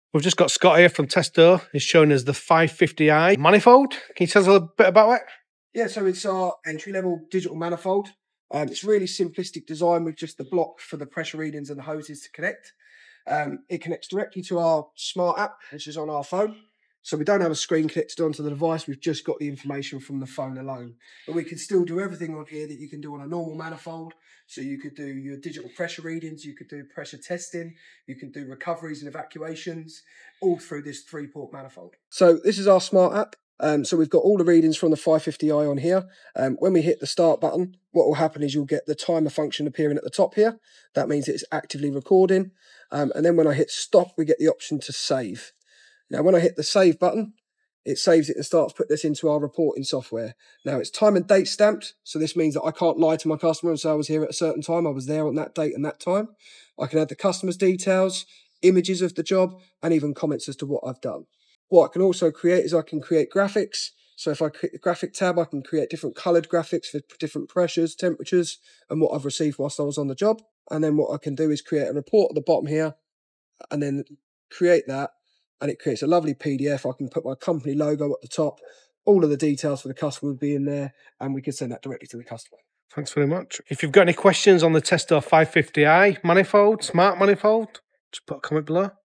Testo 550i smart manifold
The Testo 550i smart manifold is seriously impressive. Scott from Testo gave us the rundown, and here's why this tool is a game-changer:

Wireless: It's an entry-level digital manifold that connects directly to your phone via the Testo Smart App. No more clunky screen on the device itself.

Complete Functionality: You can still do everything you'd do with a traditional manifold, including digital pressure readings, pressure testing, recoveries, and evacuations.

Easy Reporting: The app actively records your measurements with a timer function and allows you to save and create reports with time and date stamps, customer details, and even images.

Professional Output: You can create customizable PDFs with your company logo and send them directly to the customer.

It's all about working smarter, not harder. Let me know what you think of this tool in the comments!